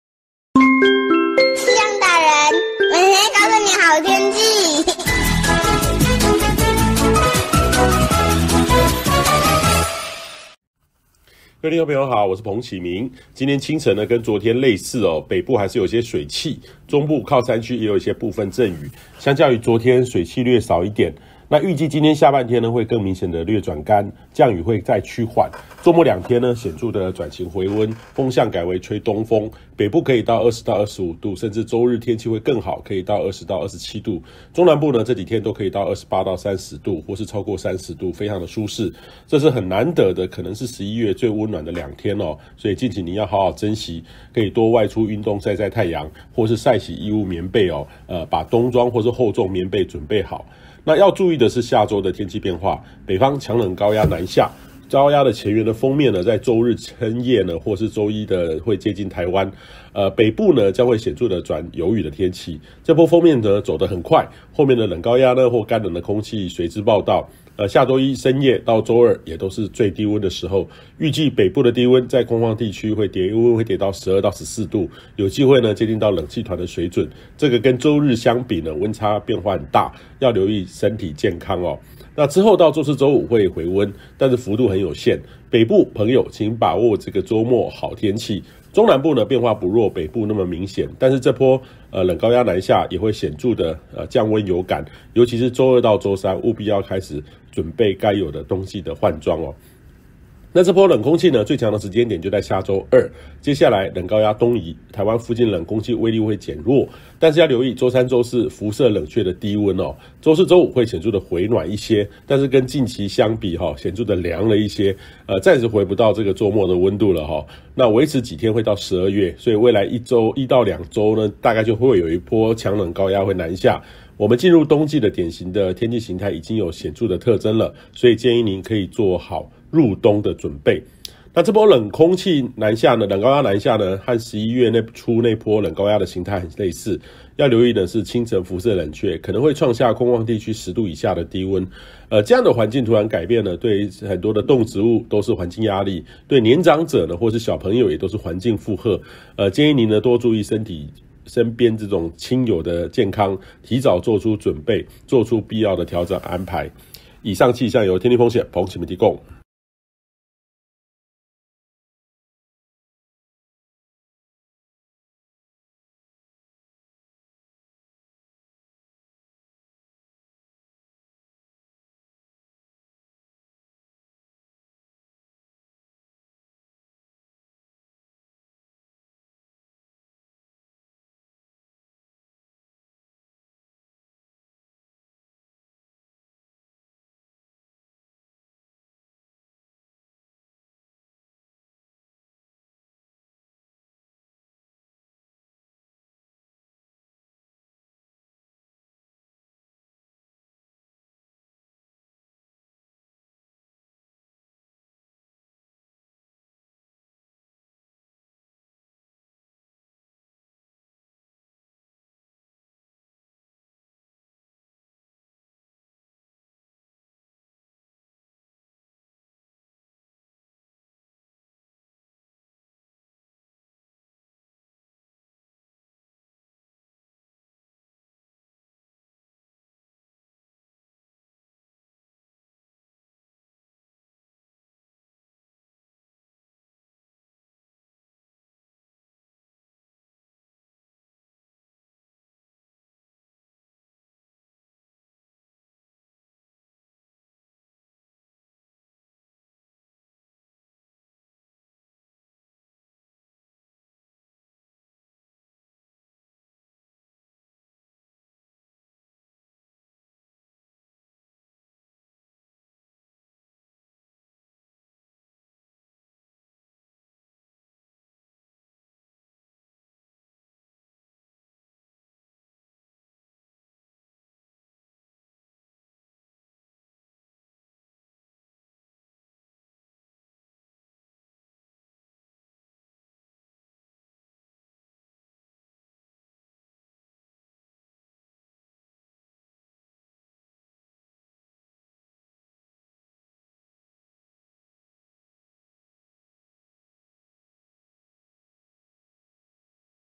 把握周末回溫轉晴好天氣 下周一強冷高壓南下
今天清晨和昨天類似，北部仍有些水氣，中部靠山區也有部分陣雨。相較於昨天水氣略少一些，預計今天下半天會更明顯的略轉乾，降雨會再趨緩。周末兩天顯著轉晴回溫，風向改為吹東風，北部可以到20-25度，甚至周日天氣更好，可以到20-27度，中南部這兩天都可以到28-30度，或是超過30度，非常的舒適，這是很難得可能就是11月最溫暖的兩天，敬請您要好好珍惜。可以多外出運動曬曬太陽，或是曬洗衣物棉被，把冬裝或是厚重棉被準備好。

要注意下周的天氣變化，北方強冷高壓南下，高壓前緣的鋒面在周日深夜或到周一接近台灣，北部將顯著轉有雨天氣，中南部也有短暫陣雨，這波鋒面走得很快，後面的冷高壓的乾冷空氣隨之報到，下周一深夜到周二會是最低溫的時候，預計北部的低溫，在空曠區域的低溫跌到12-14度，有機會接近到冷氣團的水準，這和周日相比，溫差變化大，要注意身體健康，之後到周四五會回溫，但幅度有限，北部朋友請把握這個周末好天氣。中南部變化不若北部明顯，但這波冷高壓南下，也會顯著降溫有感，尤其周二到周三，務必要開始準備該有的冬季換裝。

這波冷空氣最強時間點就在下周二，接下來冷高壓東移，台灣附近冷空氣威力會減弱，但要留意周三四輻射冷卻的低溫，周四五會有顯著回暖一些，但和近期相比顯著的涼了一些，暫時回不到這周末的溫度，維持幾天會到12月。未來都是一到兩周有一波較強冷高壓南下，我們進入冬季的典型天氣型態已經有顯著特徵，建議您可以做好入冬的準備。

這波冷高壓南下，和11月初那波冷高壓型態很類似，要留意是清晨輻射冷卻，可能會創下空曠區域十度以下低溫，這樣的環境突然改變，對動植物都是環境壓力，對年長者或是小朋友也都是環境負荷，建議您多注意身邊親友的健康，提早做出準備，做出必要的調整安排。

以上氣象由天氣風險/彭啟明博士提供